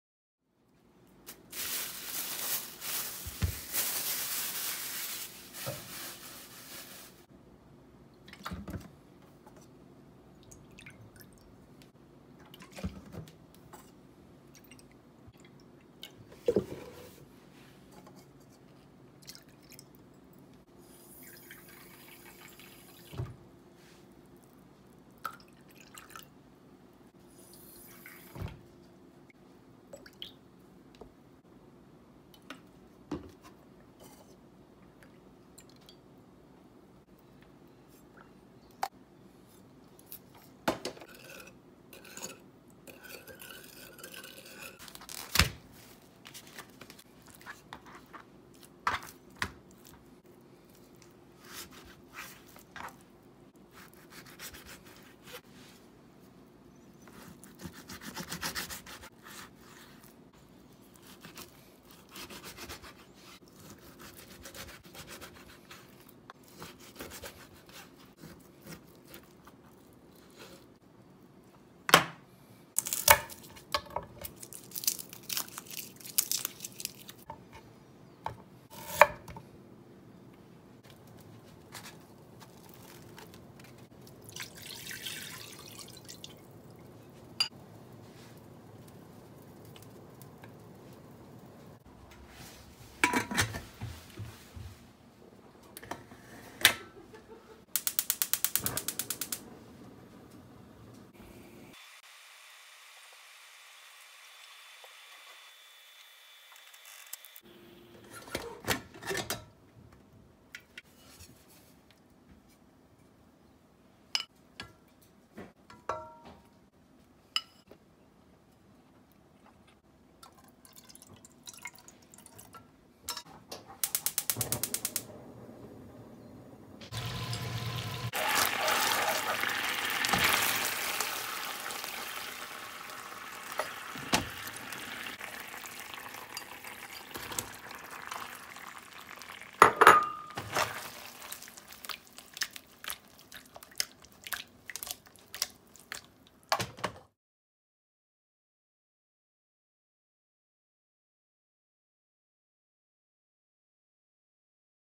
《お弁当おかずbento dishes》鶏のほろほろ煮〜圧力鍋で柔らかく
圧力鍋で鶏モモ肉を柔らかく煮ます。
甘辛醤油味です。

【具】
鶏モモ肉

【味付け】
にんにく
生姜
醤油
酒
水
みりん
砂糖